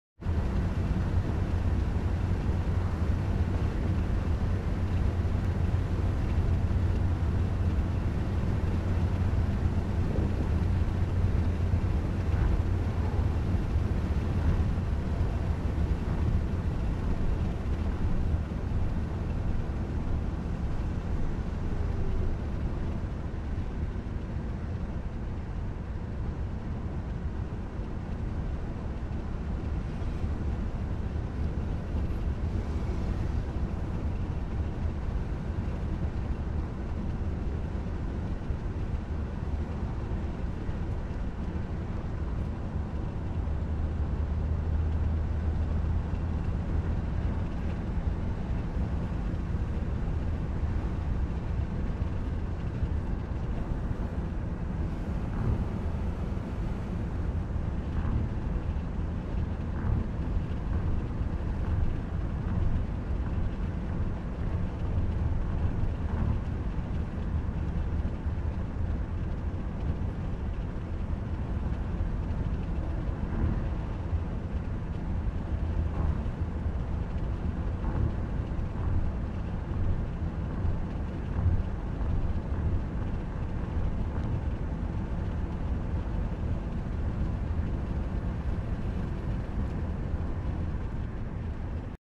786 Km Uzun Yol - Peugeot Partner Tepee - Kale / Malatya
Nokia 808 PureView

Peugeot Partner Tepee 1.6 HDi 110hp Premium Pack